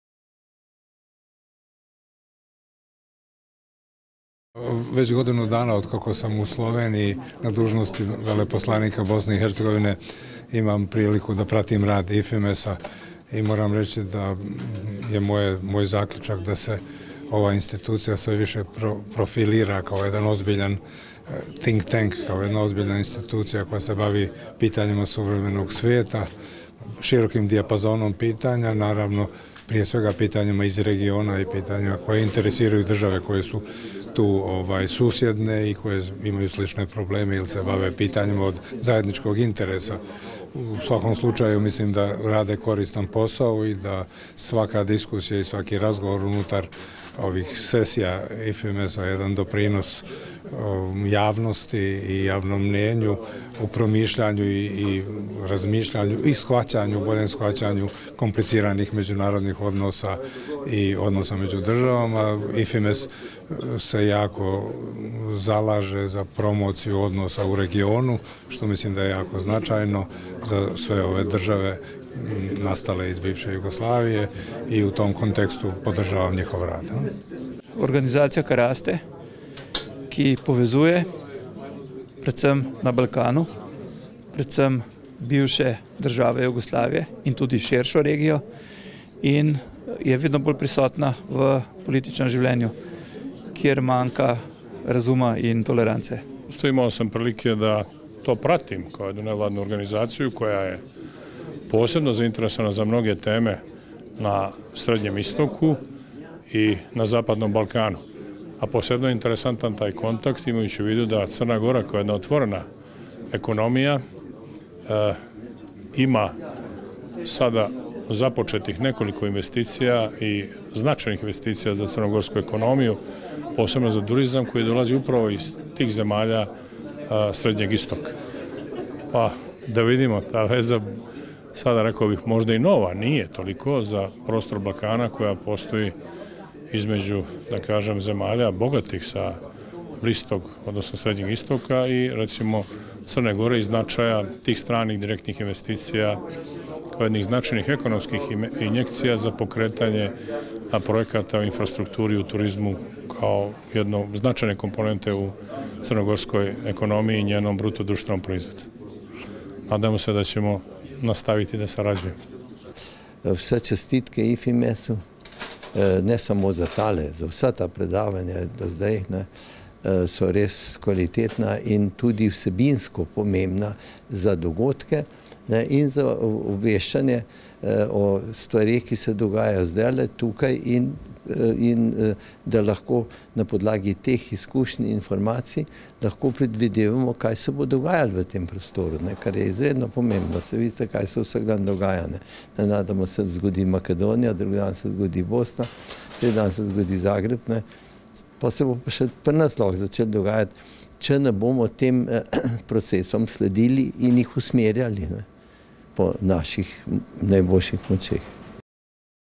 Rekli su o IFIMES/Povedali so o IFIMES - predavanje Budimir Lončar 18-5-2015
Povedali so o IFIMES - ob predavanju Budimirja Lončarja,
Ljubljana, 18. maj 2015

Željko Jerkić, Miomir Mugoša, Andrej Ribič, Božo Dimnik

Međunarodni institut za bliskoistočne i balkanske studije (IFIMES)

vas u okviru ciklusa predavanja "EU i svijet"

sa zadovoljstvom poziva na predavanje

Budimira Lončara,

predsjednika Savjetodavnog odbora Međunarodnog instituta IFIMES

sa naslovom

»Aktualni izazovi u suvremenim međunarodnim odnosima«

u ponedjeljak 18. maja/svibnja 2015, u 16.00 sati,
u hotelu »Lev«, 13. sprat/kat, dvorana »Grad«, Vošnjakova 1, Ljubljana.

Budimir Lončar (92) je donedavno bio predsjednik Savjeta za vanjsku politiku i međunarodne odnose predsjednika Republike Hrvatske dr. Ive Josipovića i posebni savjetnik predsjednika Republike Hrvatske Stjepana Mesića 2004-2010. U svom predavanju govorit će o aktualnim izazovima u suvremenim međunarodnim odnosima.

Budimir Lončar je bio zadnji ministar vanjskih poslova SFRJ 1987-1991. Između ostaloga bio je specijalni izaslanik Generalnoga sekretara UN-a pri Pokretu nesvrstanih, veleposlanik SFRJ u Washingtonu, Bonnu i Džakarti. Potpredsjednik je Svjetske asocijacije bivših ministara vanjskih poslova i predsjednik Diplomatskog vijeća Visoke škole za međunarodne odnose i diplomaciju »Dag Hammarskjöld«.

Predavanje će biti na hrvatskom jeziku,  poslije predavanja slijedi razgovor sa gostom, kojeg će voditi mr. sc.  Zijad Bećirović.

Molimo vas, da sjedišta u dvorani zauzmete najkasnije do 15.55.

Sudjelovanje na događaju je besplatno, ipak vas molimo, da svoju prisutnost obavezno potvrdite na telefonskom broju odnosno e-mail

Ljubljana, 13.maj 2015

Međunarodni institut za bliskoistočne
i balkanske studije (IFIMES) – Ljubljana
Vošnjakova ulica 1, p.p. 2795, SI-1001 Ljubljana